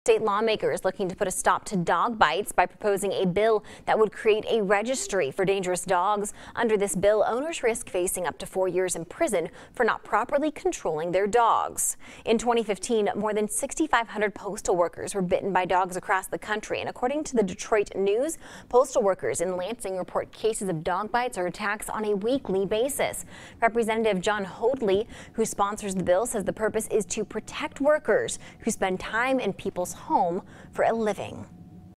New bill would create dog registry
A state lawmaker is looking to put a stop to dog bites by proposing a bill that would create a registry for dangerous dogs. Under this bill, owners risk facing up to four years in prison for not properly controlling their dogs.